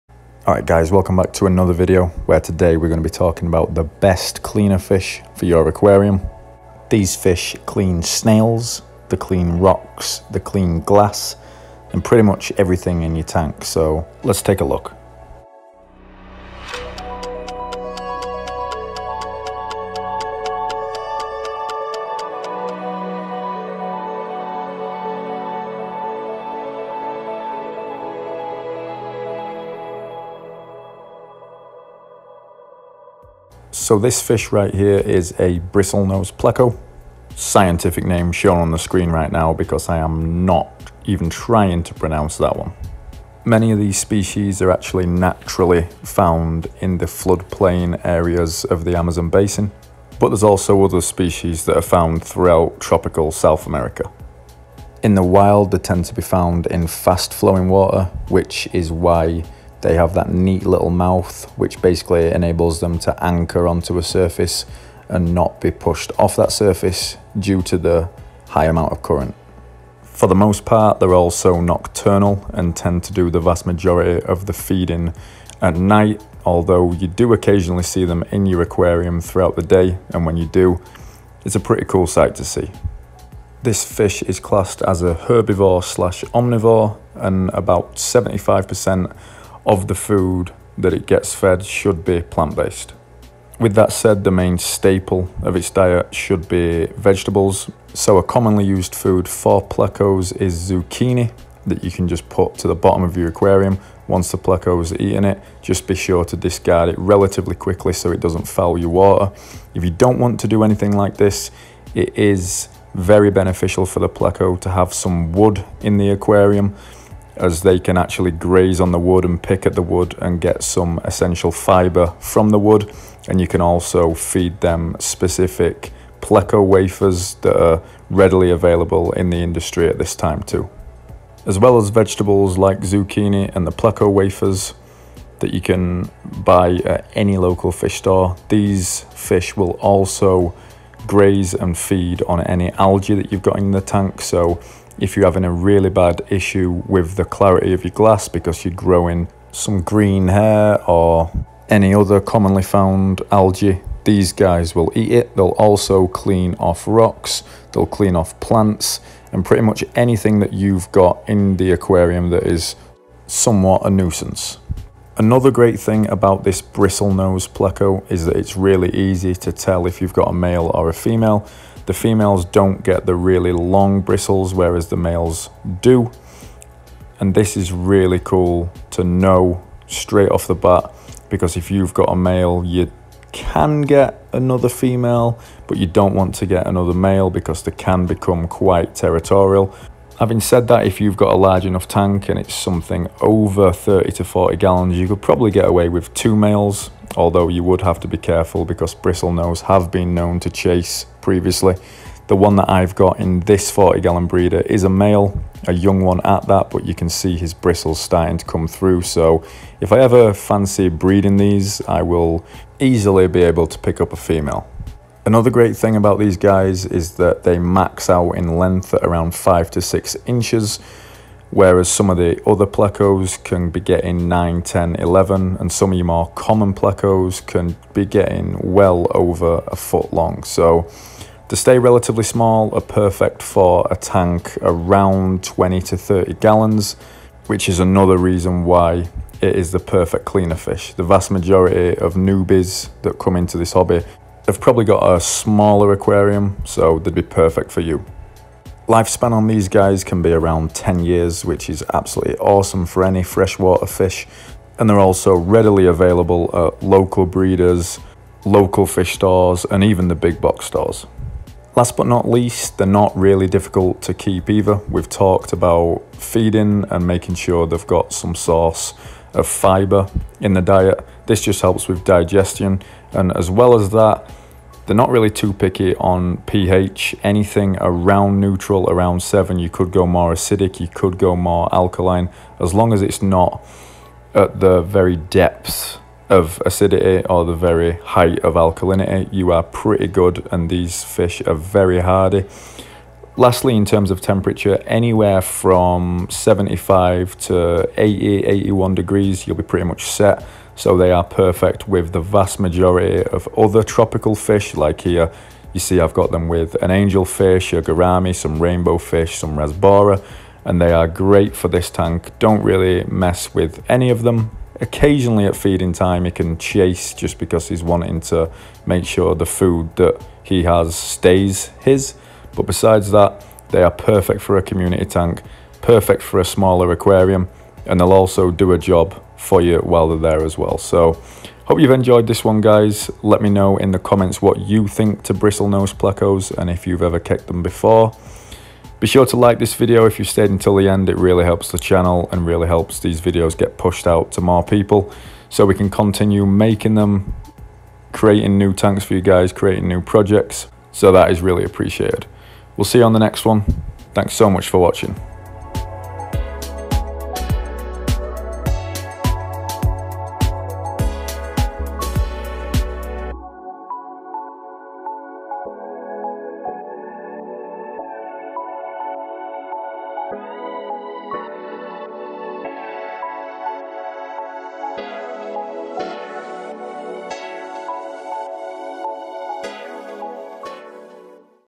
BRISTLENOSE PLECO | species spotlight & care guide | THE BEST CLEANER FISH FOR YOUR AQUARIUM!
Today we take a look at the best cleaner fish for your aquarium/fishtank & talk about the reasons why!

THE BRISTLENOSE PELCO!
Location- South America
Max size- 5-6 inches (females smaller)
PH- 6.5-7.5 (they are a hardy fish though so lower or higher would be ok)
Food to Feed- 75% veggies, 25% protein. (specific "pleco wafers" are offered on the market)
Minimum tank size- 20-30 gallons
Difference in male & female- Male's have the big "bristles" on their face

Thank you for allowing me to use this footage!

Sponge filter

Whisper air pumps

aquaclear filter

Asta 20 light

aqueon pro heater

Ada aquasoil

Current light

Finnex 24/7